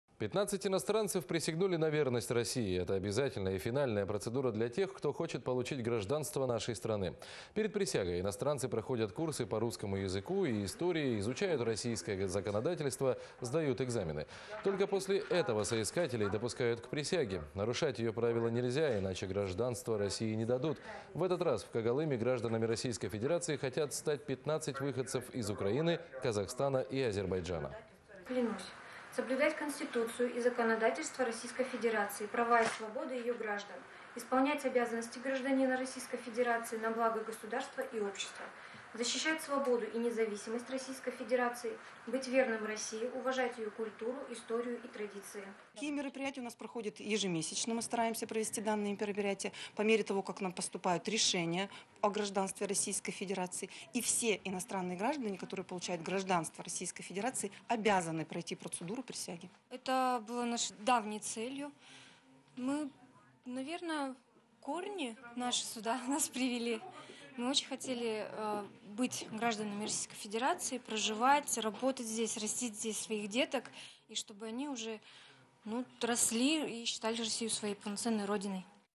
15 иностранцев в Когалыме стали гражданами России
15 иностранцев присягнули на верность России. Это обязательная и финальная процедура для тех, кто хочет получить гражданство нашей страны. Перед присягой иностранцы проходят курсы по русскому языку и истории, изучают российское законодательство, сдают экзамены. Только после этого соискателей допускают к присяге. Нарушать её правила нельзя, иначе гражданство России не дадут.